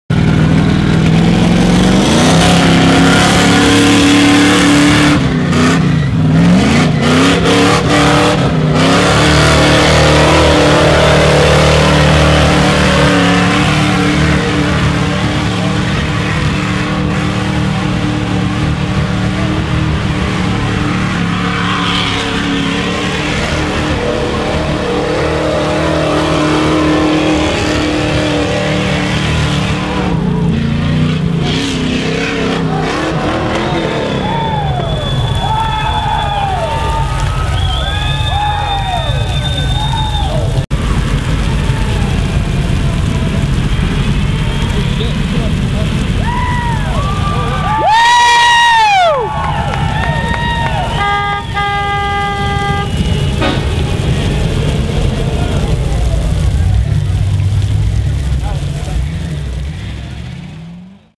BIG BLOCK MUD TRUCK ROLLOVER
Steveo rolls his Big Block powered Ford truggy in the mud pit at Great american Park in auburn Alabama. This took place during the first annual Rockfest event. MUD ROLLOVER